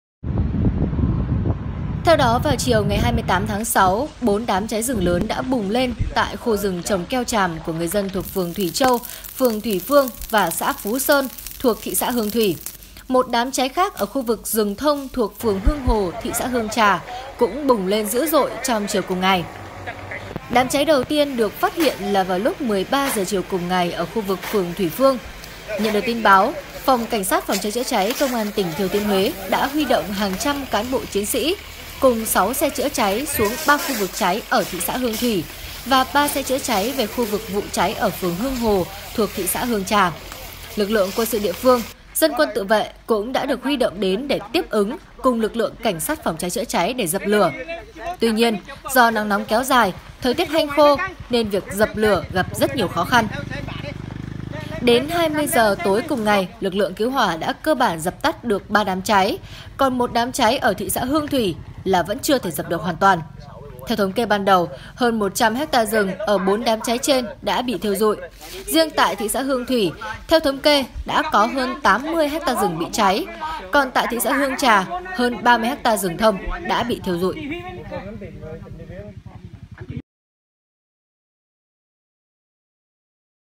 Thừa Thiên - Huế: Rừng cháy lớn cùng lúc ở 4 khu vực
Tầm nhìn TV: Trong 1 ngày, Thừa Thiên - Huế xảy ra 3 vụ cháy rừng lớn trong nhiều giờ. Cơ quan chức năng tỉnh này đang huy động nhiều lực lượng ứng phó.